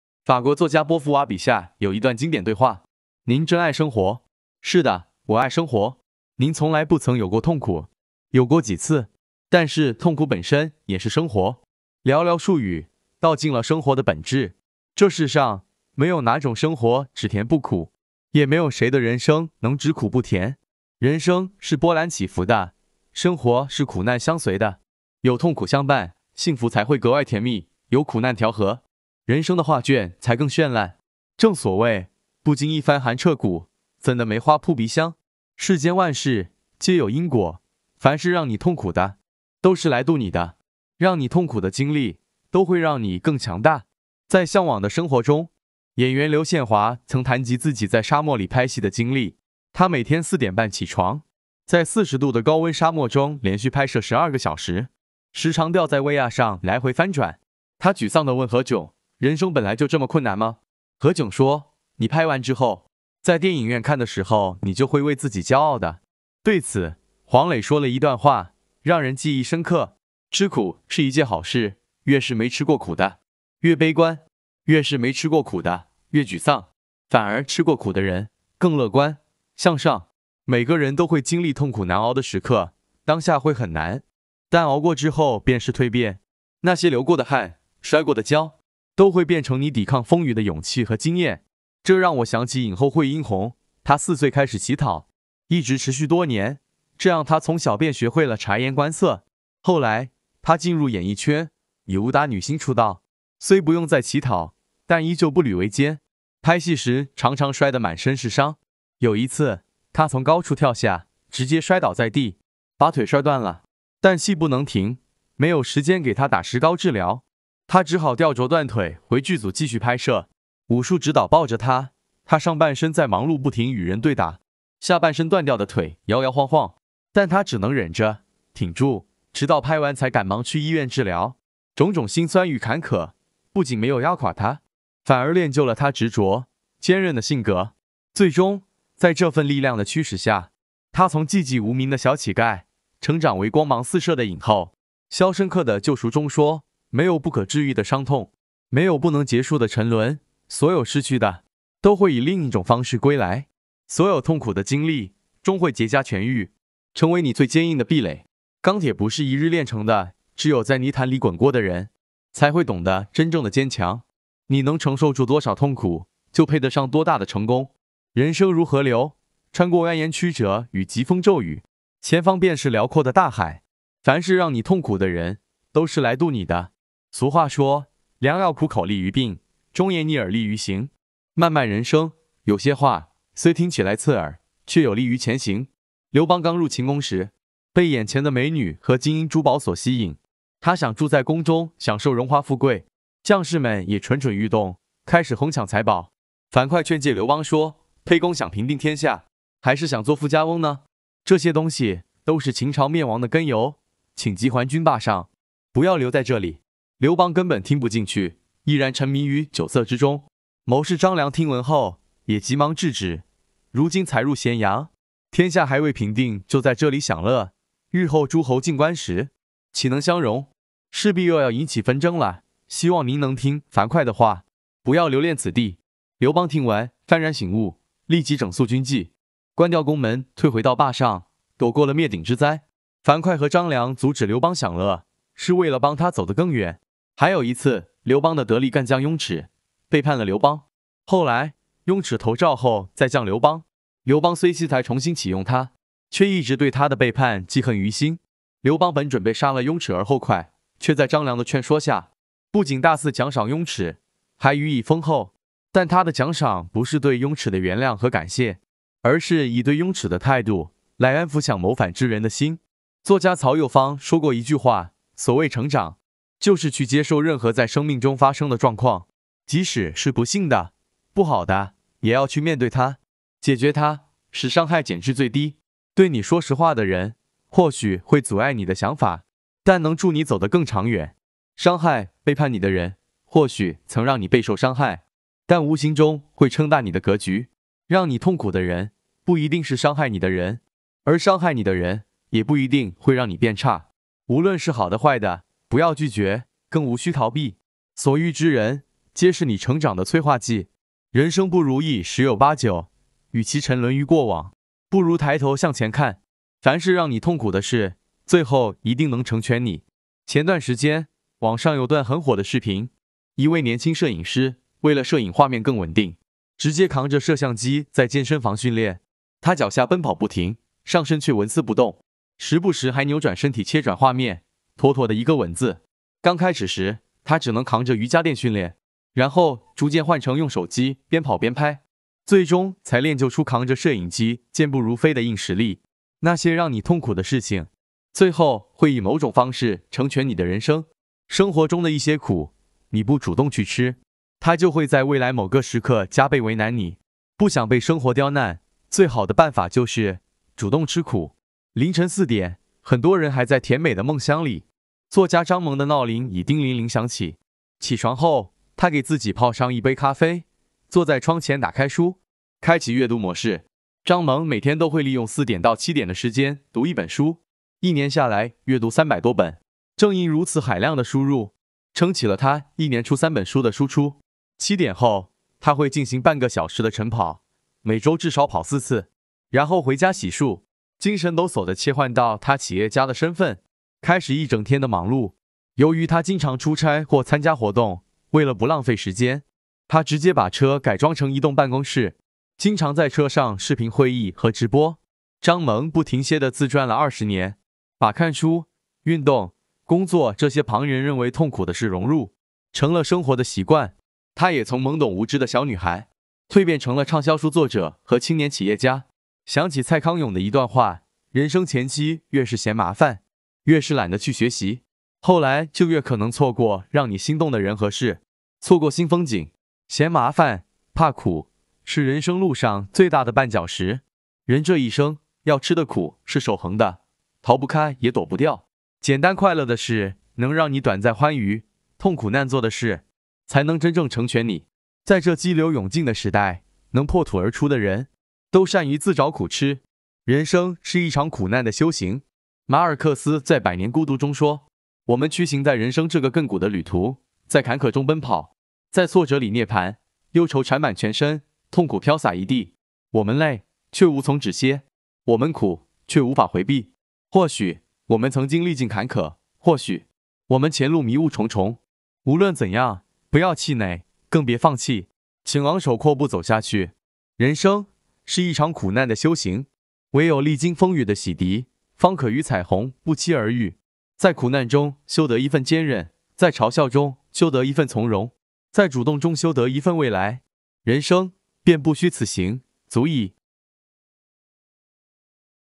凡是让你痛苦的，都是来渡你的
还有一次，刘邦的得力干将雍齿，背叛了刘邦。
后来，雍齿投赵后再降刘邦，刘邦虽惜才重新启用他，却一直对他的背叛记恨于心。
刘邦本准备杀了雍齿而后快，却在张良的劝说下，不仅大肆奖赏雍齿，还予以封候。
但他的奖赏不是对雍齿的原谅和感谢，而是以对雍齿的态度，来安抚想谋反之人的心。
作家曹又方说过一句话:“所谓成长，就是去接受任何在生命中发生的状况，即使是不幸的，不好的，也要去面对它，解决它，使伤害减至最低。”
对你说实话的人，或许会阻碍你的想法，但能助你走得更长远。
伤害背叛你的人，或许曾让你备受伤害，但无形中会撑大你的格局。
让你痛苦的人，不一定是伤害你的人，而伤害你的人，也不一定会让你变差。
无论是好的坏的，不要拒绝，更无须逃避，所遇之人，皆是你成长的催化剂。
人生不如意十有八九，与其沉沦于过往，不如抬头向前看。

凡是让你痛苦的事，最后一定能成全你。
前段时间，网上有段很火的视频:一位年轻摄影师，为了摄影画面更稳定，直接扛着摄像机在健身房训练。
他脚下奔跑不停，上身却纹丝不动，时不时还扭转身体切转画面，妥妥地一个“稳”字。
刚开始时，他只能扛着瑜伽垫训练，然后逐渐换成用手机边跑边拍，最终才练就出扛着摄影机健步如飞的硬实力。
那些让你痛苦的事情，最后会以某种方式成全你的人生。
生活中的一些苦，你不主动去吃，它就会在未来某个时刻加倍为难你。
不想被生活刁难，最好的办法就是，主动吃苦。
凌晨4点，很多人还在甜美的梦乡里，作家张萌的闹铃已叮铃铃响起。
起床后，她给自己泡上一杯咖啡，坐在窗前打开书，开启阅读模式。
张萌每天都会利用4点到7点的时间读一本书，一年下来阅读300多本。
正因如此海量的输入，撑起了她一年出3本书的输出。
7点后，她会进行半个小时的晨跑，每周至少跑4次。
然后回家洗漱，精神抖擞地切换到她企业家的身份，开始一整天的忙碌。
由于她经常出差或参加活动，为了不浪费时间，她直接把车改装成移动办公室，经常在车上视频会议和直播。
张萌不停歇地自转了20年，把看书、运动、工作这些旁人认为痛苦的事融入成了生活的习惯。
她也从懵懂无知的小女孩，蜕变成了畅销书作者和青年企业家。
想起蔡康永的一段话:“人生前期越是嫌麻烦，越是懒得去学习，后来就越可能错过让你心动的人和事，错过新风景。”
嫌麻烦、怕苦，是人生路上最大的绊脚石。
人这一生，要吃的苦是守恒的，逃不开也躲不掉。
简单快乐的事，能让你短暂欢愉，痛苦难做的事，才能真正成全你。
在这激流勇进的时代，能破土而出的人，都善于“自找苦吃”。

人生是一场苦难的修行。
马尔克斯在《百年孤独》中说:“我们趋行在人生这个亘古的旅途，在坎坷中奔跑，在挫折里涅槃，忧愁缠满全身，痛苦飘洒一地。
我们累，却无从止歇;
我们苦，却无法回避。”
或许，我们曾经历尽坎坷，或许，我们前路迷雾重重。
无论怎样，不要气馁，更别放弃，请昂首阔步走下去。
人生，是一场苦难的修行。
唯有历经风雨的洗涤，方可与彩虹不期而遇。
在苦难中修得一份坚韧，在嘲笑中修得一份从容，在主动中修得一份未来。
人生，便不虚此行，足矣。